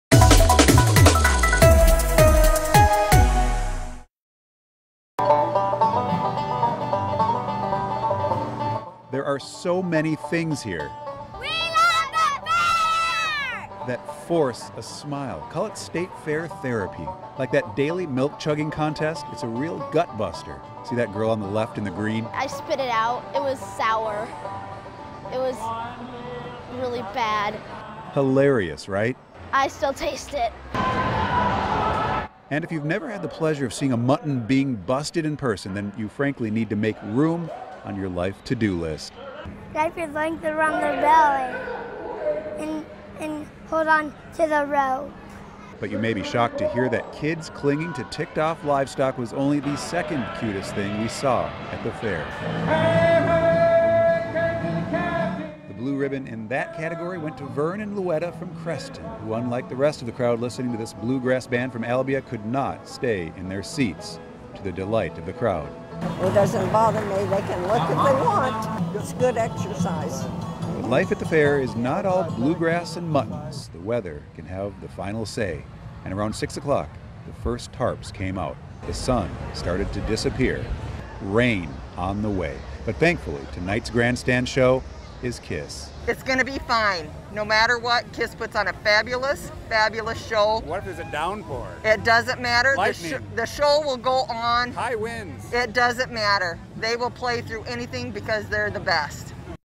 A fun-filled Friday at the state fair!
Rain didn't stop fairgoers from enjoying the last Friday of the fair.